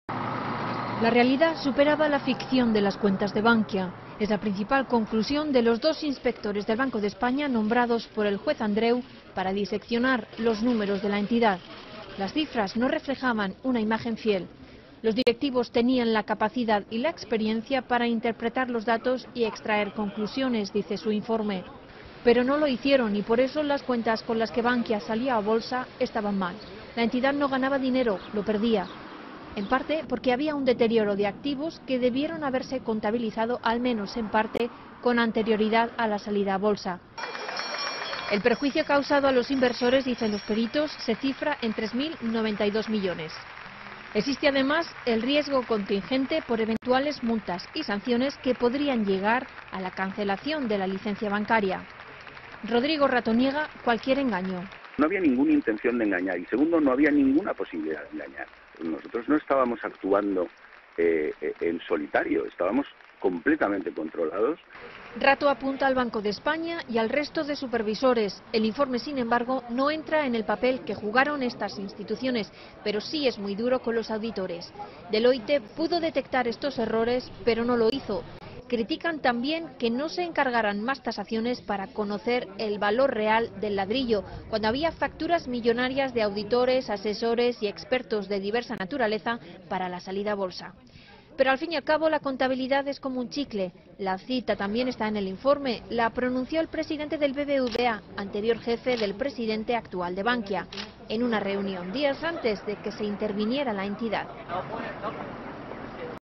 La realidad superaba la ficción de las cuentas de Bankia según un informe encargado por el juez
La realidad superaba la ficción de las cuentas de Bankia. Es la principal conclusión de dos inspectores del banco de España nombrados por el juez Andreu para diseccionar los números de la entidad. "Las cifras no reflejaban una imagen fiel"  Principalmente porque no se reconocieron las pérdidas del ladrillo a tiempo. Y eso viene en gran parte de antes de la fusión de las siete cajas: "los directivos tenían la capacidad y la experiencia para interpretar los datos y extraer conclusiones". Pero no lo hicieron y por eso las cuentas con las que Bankia salió a bolsa estaban mal. La entidad no ganaba dinero, lo perdía. "Había un deterioro de activos que debió haberse contabilizado, al menos en parte, con anterioridad a la salida a bolsa", dice el informe. "El perjuicio causado a los inversores, según  los peritos, se cifra en 3.092 millones". "Existe el riesgo contingente por eventuales multas y sanciones que podrían llegar a la cancelación de la licencia bancaria", alertan los peritos. Rodrigo Rato niega cualquier engaño. "No había ninguna intención de engañar. No había ninguna posibilidad de engañar. Nosotros no actuábamos en solitario, estábamos absolutamente controlados" ha declarado al programa "Herrera en la onda". Rato apunta al Banco de España y al resto de supervisores. El informe sin embargo no entra en el papel que jugaron estas instituciones pero sí es muy duro con los auditores. Deloitte pudo detectar estos errores pero no lo hizo. Critican también que no se encargaran más tasaciones para conocer el valor real del ladrillo cuando había "facturas millonarias de auditores, asesores y expertos de diversa naturaleza" para la salida a bolsa. Pero al fin y al cabo "la contabilidad es como un chicle, y todo depende de cómo se aplique la norma". La cita también está en el informe. La pronunció el presidente del BBVA, anterior jefe del presidente actual de Bankia, en una reunión días antes de que se rescatara la entidad.